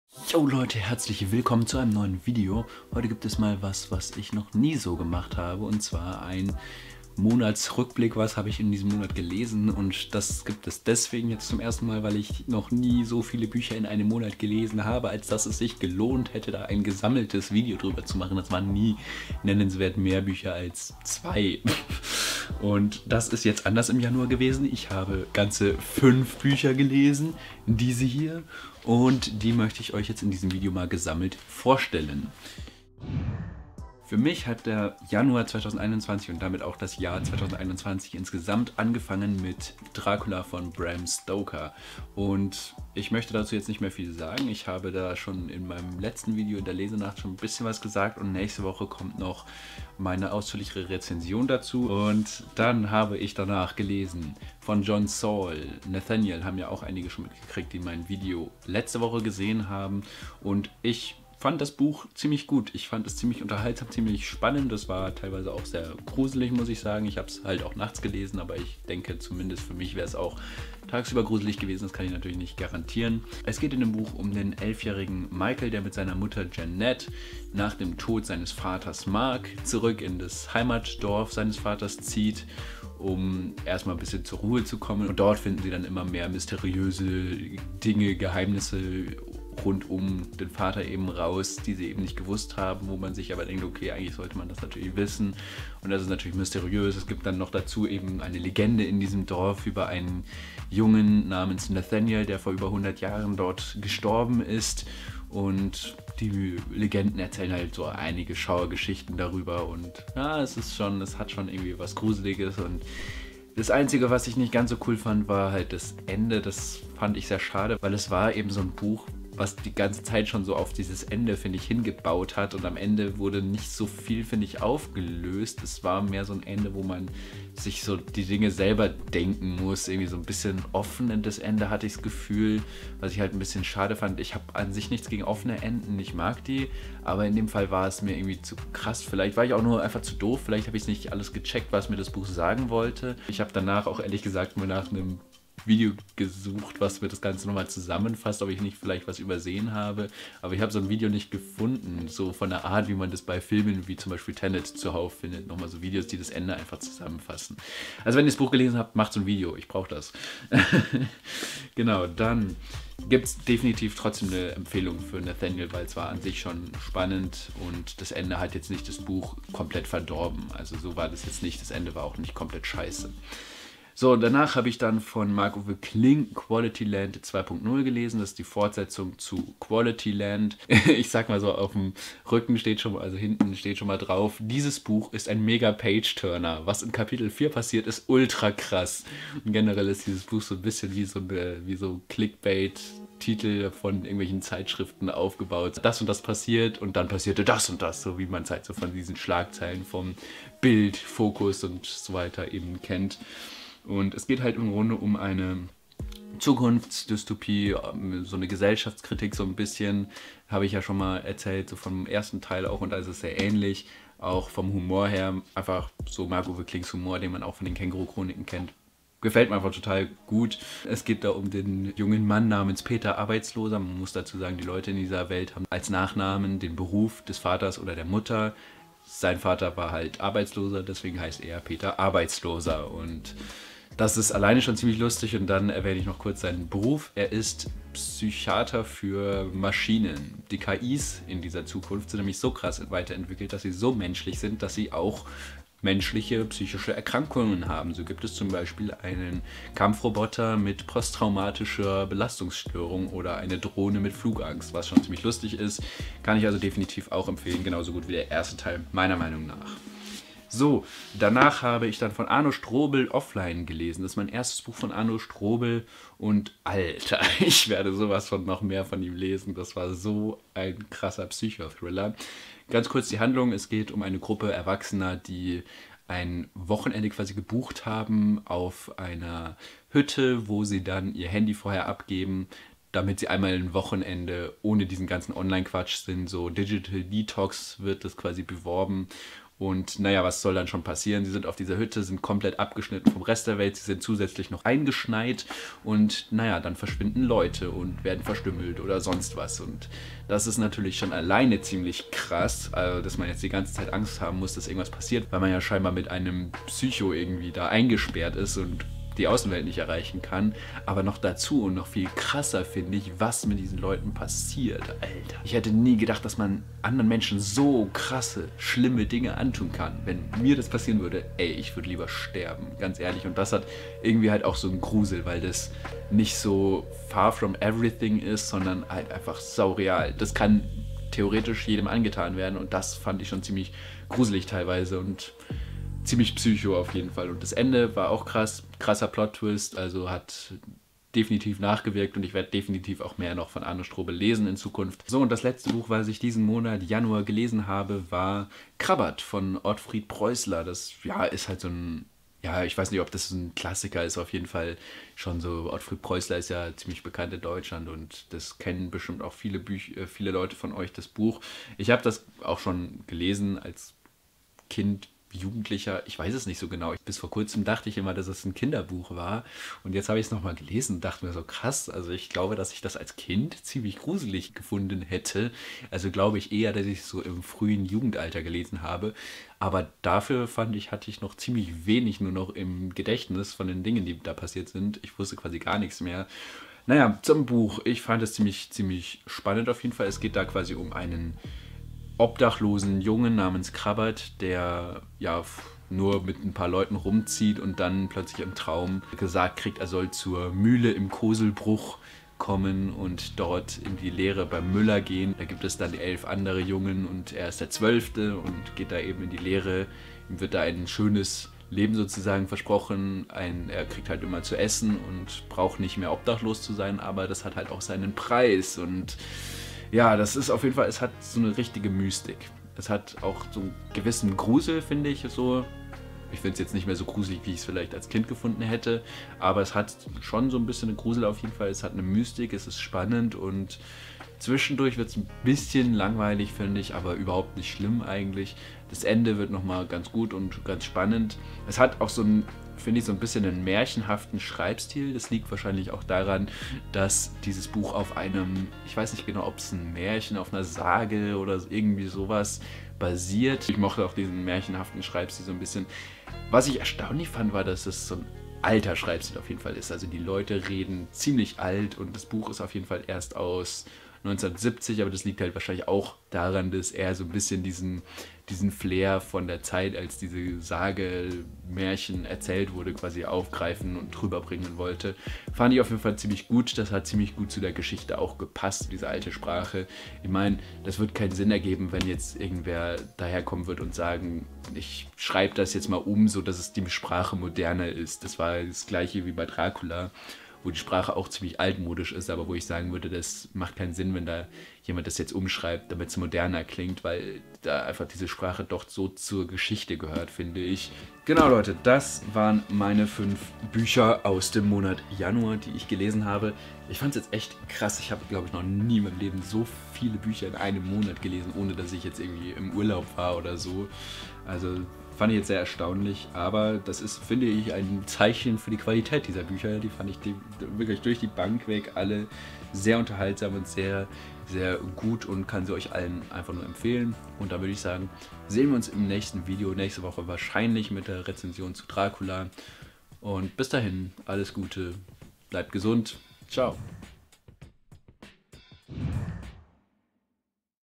Lesemonat Januar 2021
Ganze fünf Bücher habe ich im Januar gelesen. Die möchte ich euch heute vorstellen. Viel Spaß damit. :D
0:00 - Einleitung
0:34 - Dracula
0:52 - Nathaniel
2:53 - Qualityland
4:20 - Offline
5:45 - Krabat
8:21 - Schlusswort
Du magst Bücher und Geschichten genauso gerne wie ich?
Super! Denn hier gibt es BUCHREZENSIONEN, -TALKS und ab und an eine SELBSTGESCHRIEBENE KURZGESCHICHTE als Hörbuch.